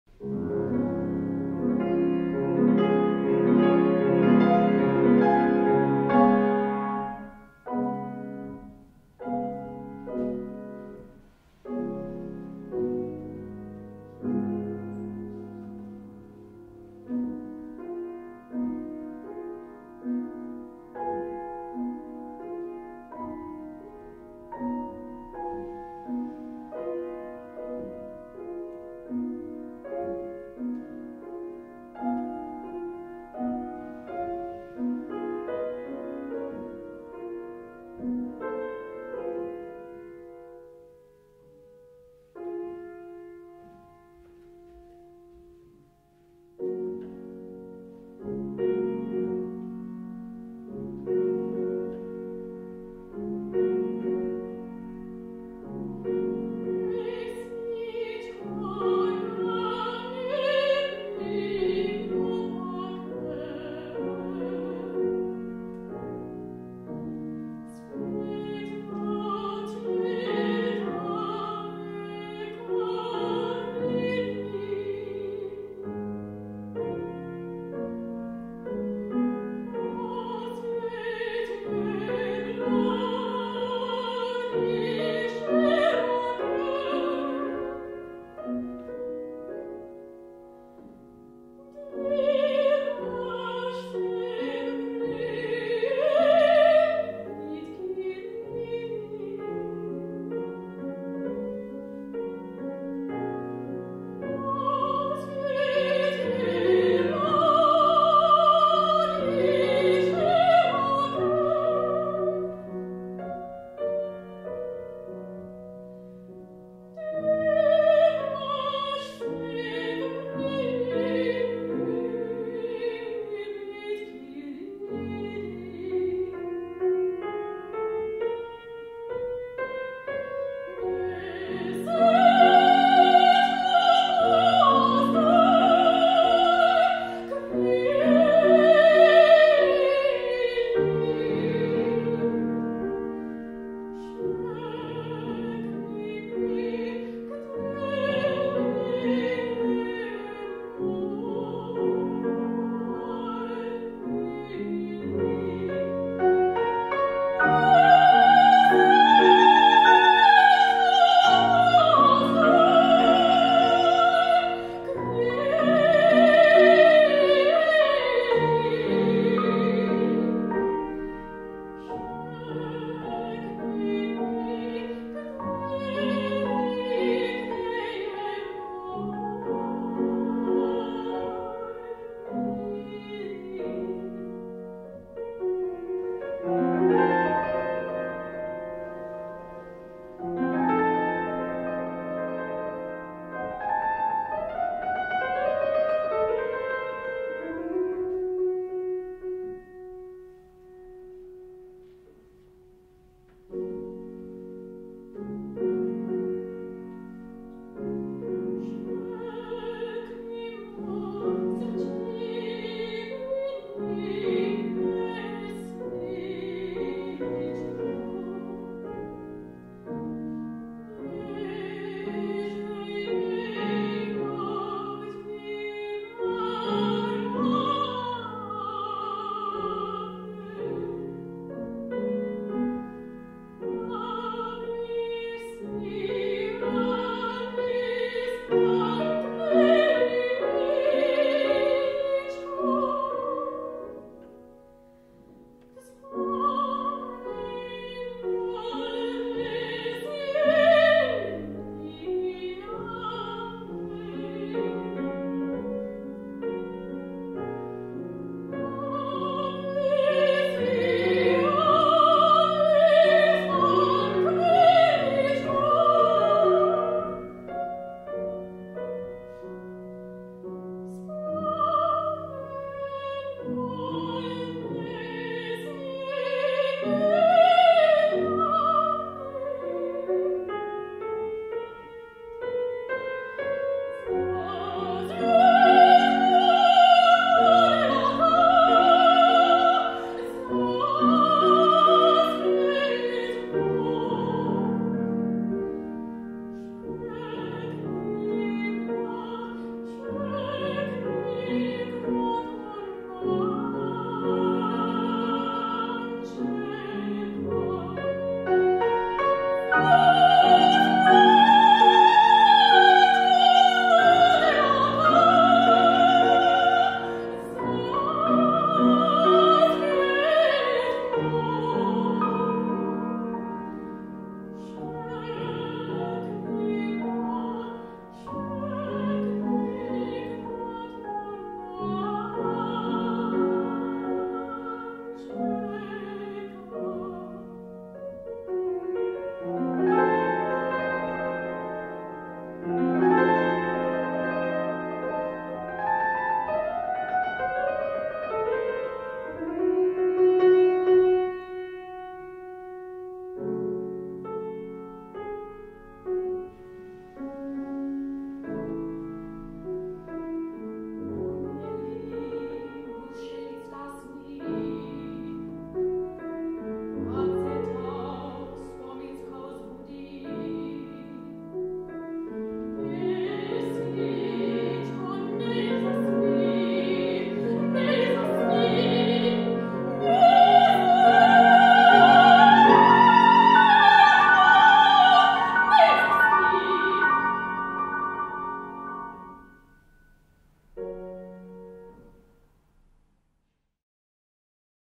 Opera Music Video #2
A music video I made this year for two friends to show off their amazing talents!
Shot on Canon C100 with 1970's Nikor 35mm/ 85mm Prime lenses